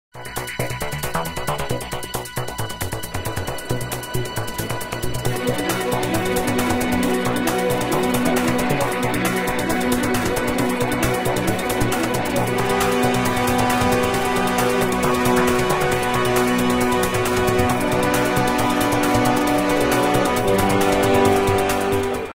JUMP | Hall 5000 Stand B21
Stand de la société JUMP, qui a exposé au salon SEPEM Douai 2015. SEPEM est LE salon des services, équipements, process et maintenance de vos régions !